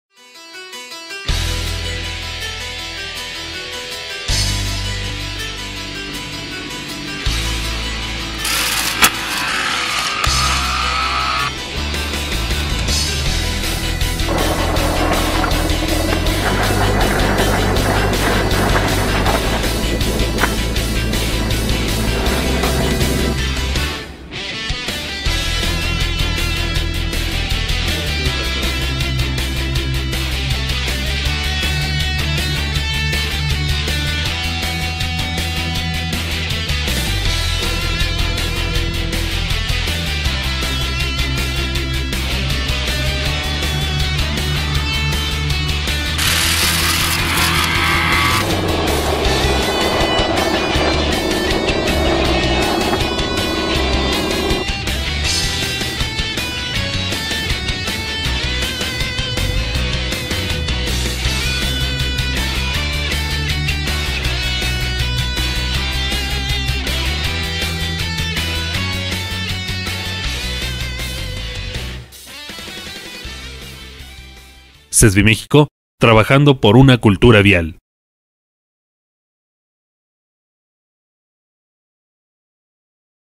Campaña de Seguridad Vial 2013: Casco
Clip que demuestra la importancia del uso del casco y demás elementos de seguridad al conducir una motocicleta.